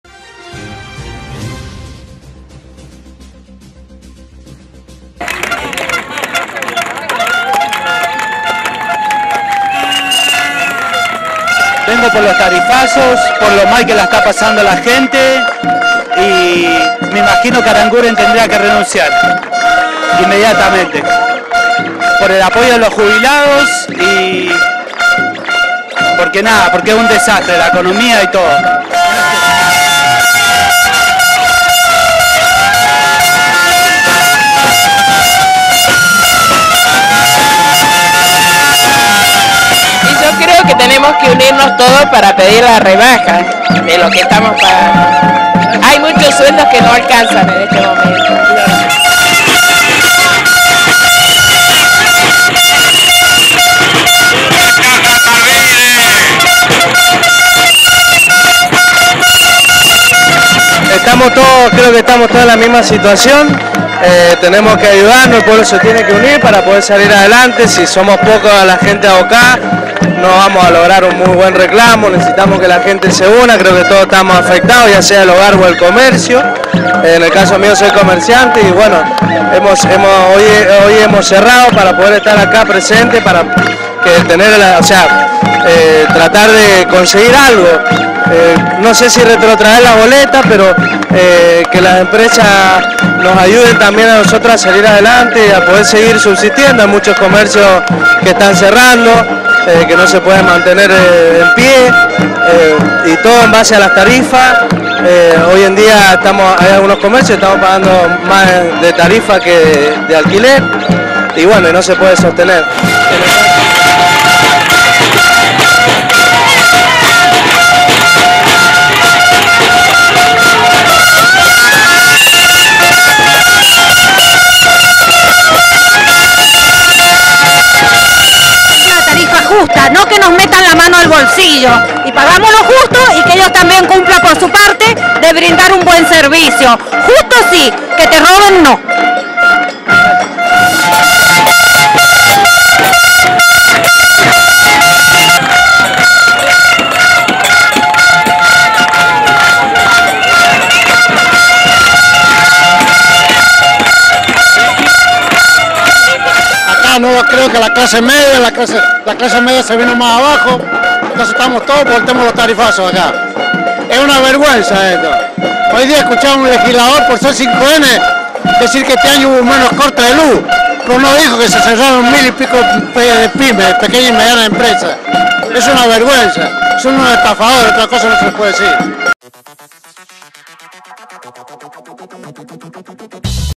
RUIDAZO SANRAFAELINO
Comerciantes, empresarios, gremialistas, trabajadores, amas de casa, ciudadanos de a pié se dieron cita en el km 0 de la ciudad de San Rafael para hacer oír sus quejas y sinsabores ante la escalada descomunal de ls tarifas de los servicios básicos, como la luz, el gas y el agua corriente.
Hay declaraciones que demuestran la angustia social.